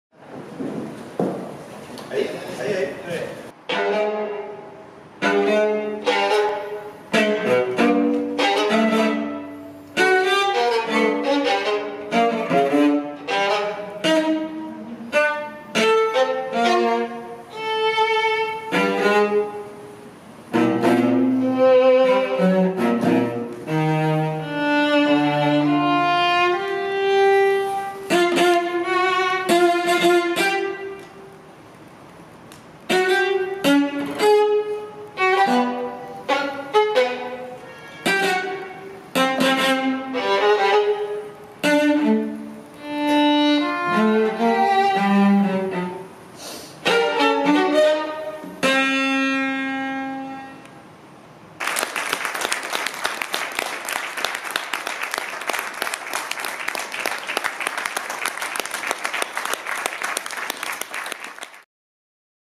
Konsert i Dome of Visions: Skärstråk
Helena Schuback och Peter Schuback framförde konserten Skärstråk i Dome of Visions i Stockholm den 20 april 2017. Film: Joakim Kröger/NCC